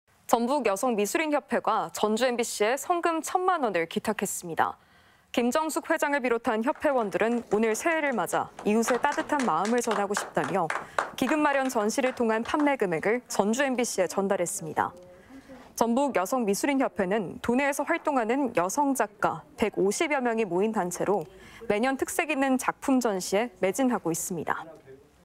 전북 여성미술인협회, 불우이웃돕기 1,000만 원 기탁 | 전주MBC 230104 방송
전북 여성미술인협회가 전주MBC에 성금 천만 원을 기탁했습니다.

김정숙 회장을 비롯한 협회원들은 오늘(4일) 새해를 맞아 이웃에 따뜻한 마음을 전하고 싶다며, 기금마련 전시를 통한 판매금액을 전주MBC에 전달했습니다.

전북 여성미술인협회는 도내에서 활동하는 여성작가 150여 명이 모인 단체로, 매년 특색있는 작품 전시에 매진하고 있습니다.